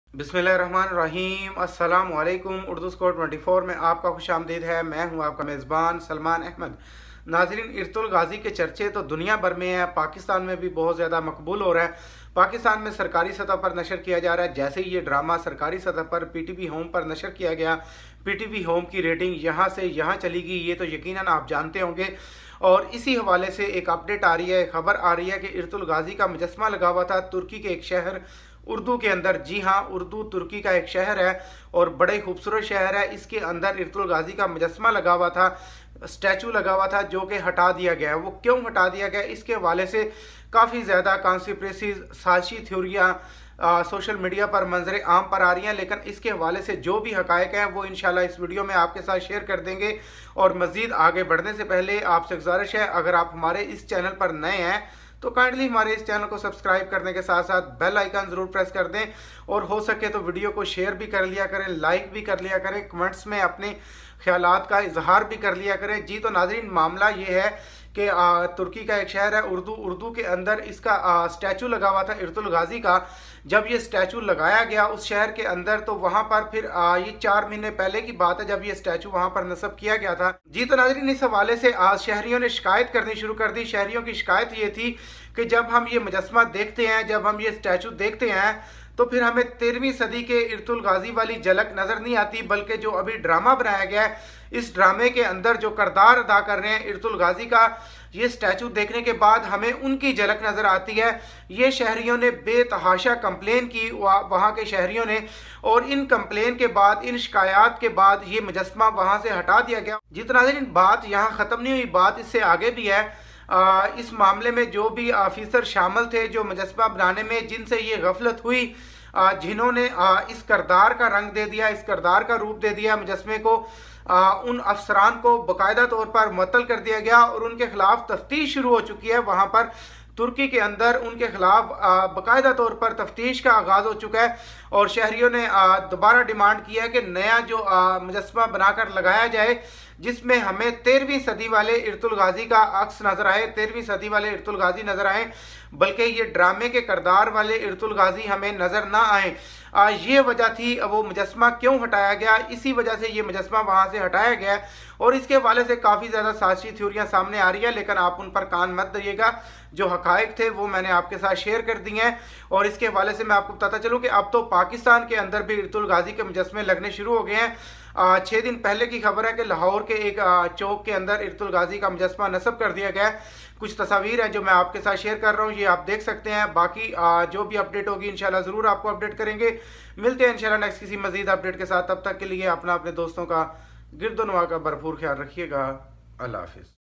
Turkey Removed Ertugurl Ghazi Statue | UrduScore24
Turkey removed Ertugurl Ghazi statue, Dirrilis Ertugurl Ghazi Statue has been removed by Turkey.